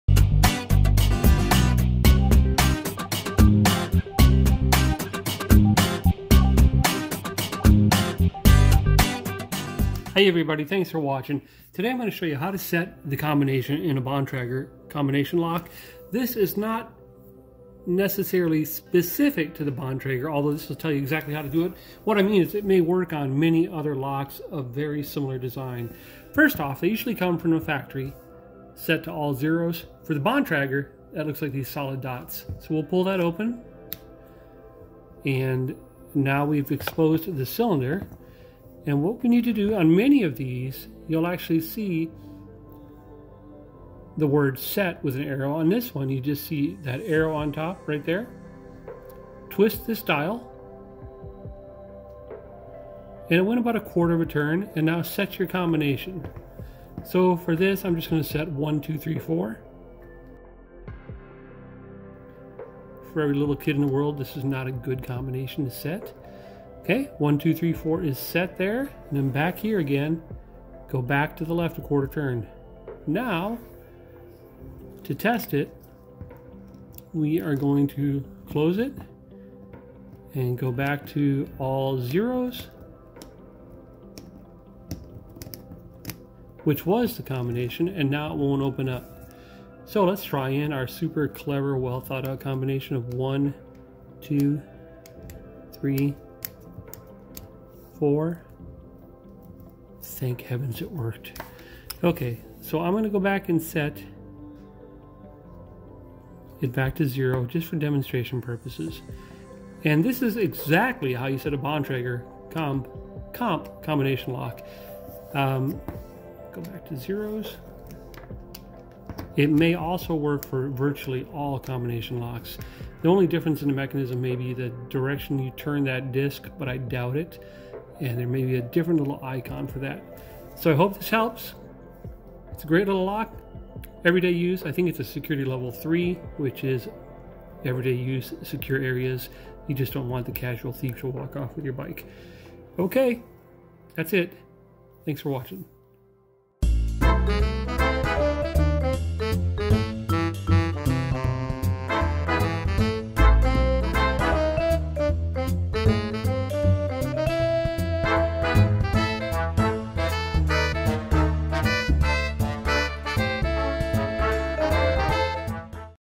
EASY DIY! How To Set A Combination on a Bike Lock (Bontrager Comp)
A quick video showing you how to EXACTLY set your own combination in a Bontrager Bike combination lock.  Works on MANY different brands.  Simple, step-by-step instructions.